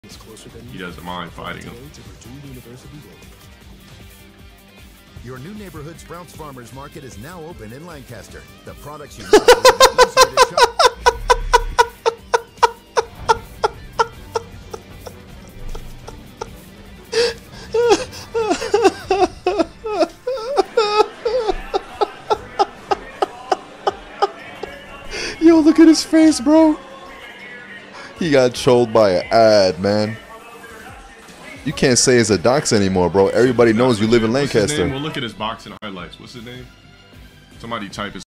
Dark DSP Variant known as Dale Wilson reveals where he lives through an ad
fucking dumbass use an adblocker you decaying piece of shit